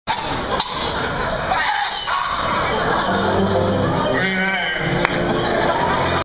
Steve Brule For Your Hair Tim and Eric Chrimbus Spectacular Seattle 2010
Video taken November 1st, 2010 in Seattle at the Showbox Sodo.